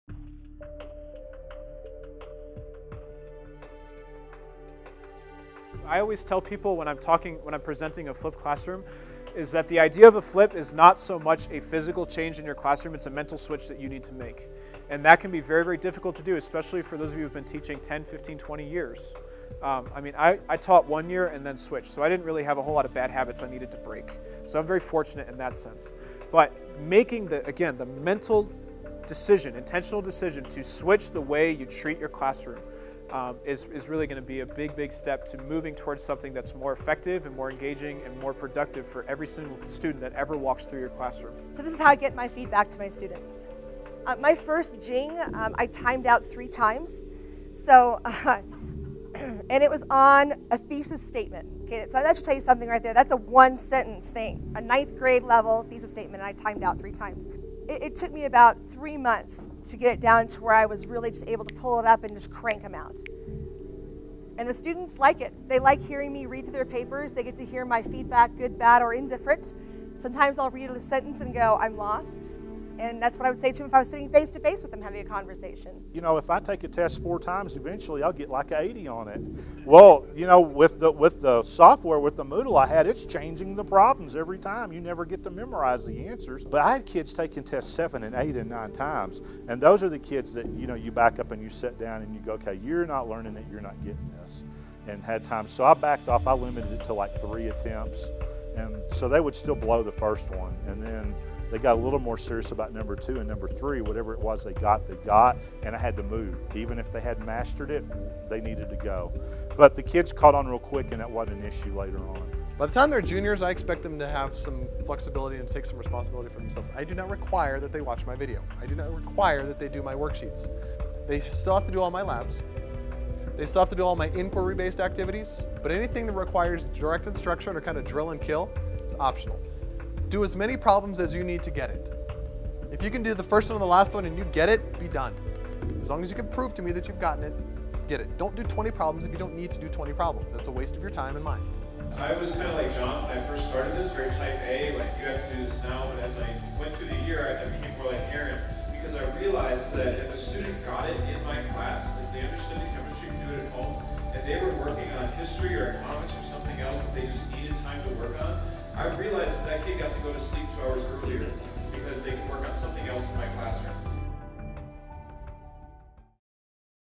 The Flipped Class-How do Teachers need to Rethink their Classes
Get a glimpse into the 2011 Flipped Class Conference.  This video addresses the question:  What is the mental switch that teachers need to make as they contemplate flipping their classes.